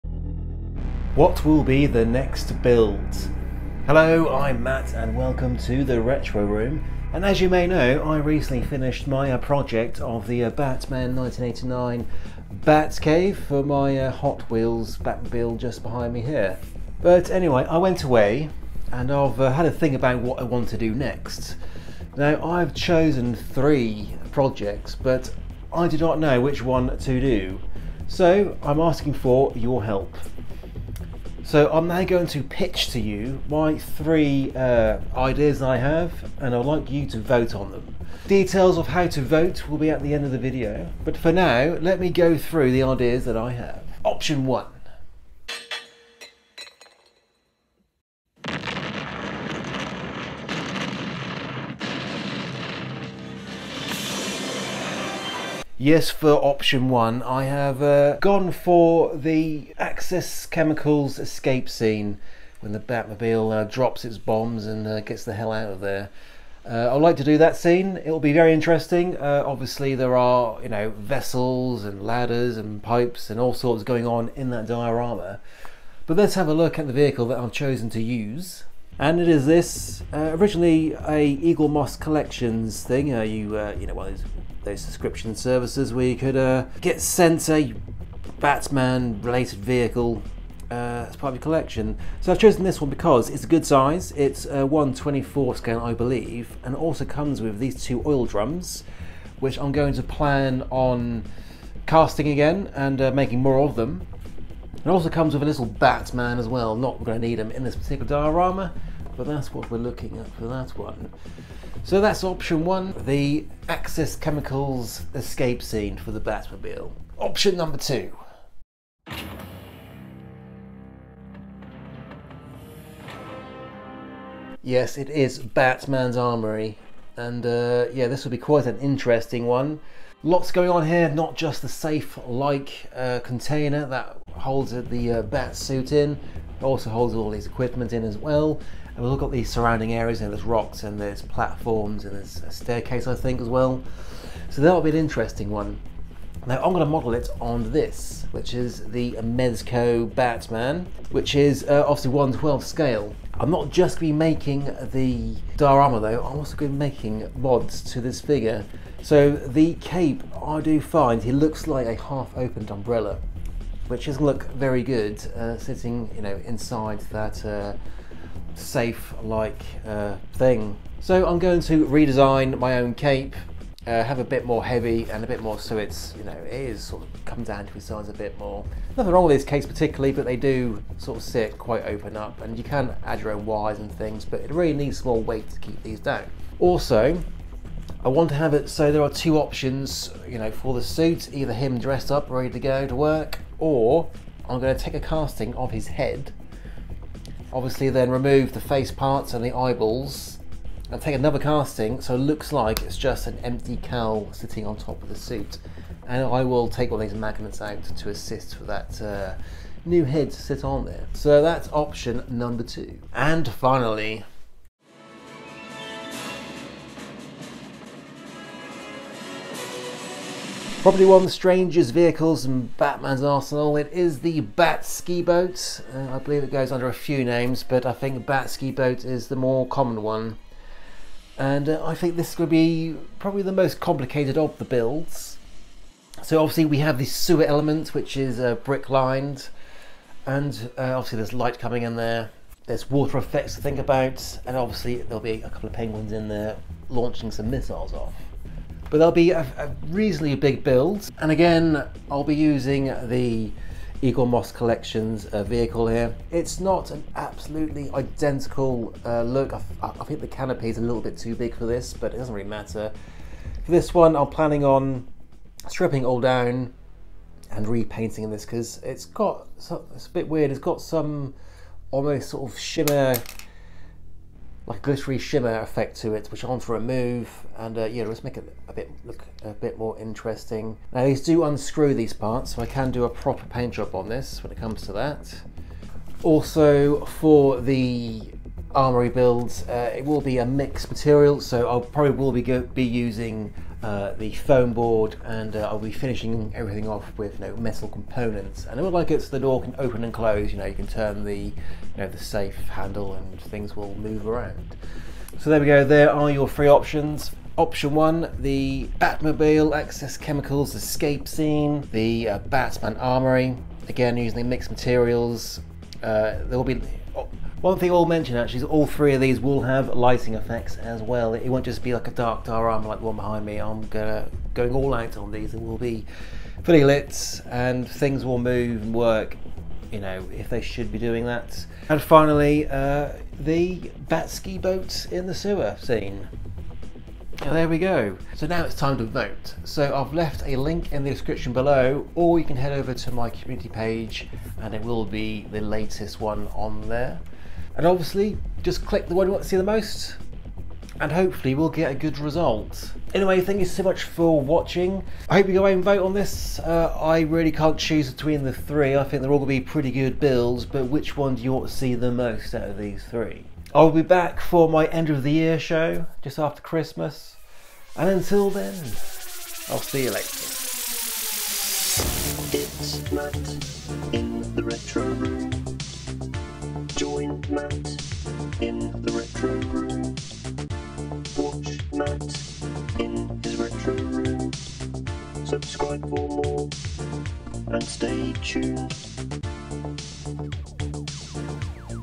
What Will Be The Next Build?
Hello and welcome to The Retro Room!

After building the Batman 1989 Batcave diorama for the 1/18 scale Hot Wheels Batmobile, I went away to think about what the next project should. I came up with 3 ideas, but I really don't know what I should do so, I NEED YOUR HELP!

I have given you 3 options,

OPTION 1... Batmobile Axis Chemicals Escape scene.

In option 1 I am planning on using a 1/24 scale Batmobile and recreate the scene where the Batmobile, after drop some bombs, races out of Axis Chemicals. This build will include lighting.

OPTION 2... Batman's Armoury.

The Mighty Keaton Batman keeps his suit and other crime fighting gadgets in a safe like container. I will be using the Mezco One:12 Keaton Batman and building the armoury and all the areas around it.
This build will also include lighting.

OPTION 3... Batskiboat

From Batman Returns it is the weird vehicle used to transit through the sewers of Gotham. This will be the biggest of the builds and will include two rocket wielding Penguins. This will also including lighting.

There are your 3 options. Link below for the voting page, which can also be accessed via the channel home page.

Thank you for your vote. Results will follow soon.

If you have any questions please use the comment section and ask away!

Outro Music by RantamA. You can find their music over on Sound Cloud